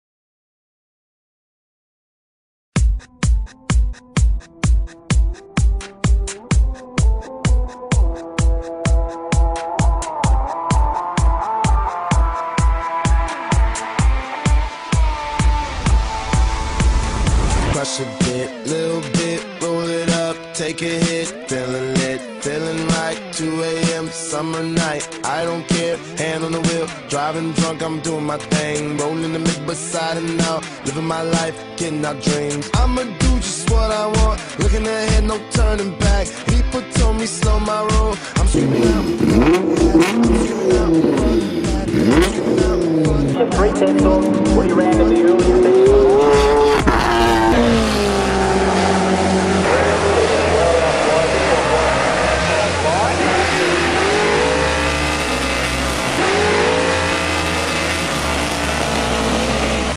WTAC vlog
Just a quick vid from 2016 WTAC shot on a Hero 5 black and lumix FZ1000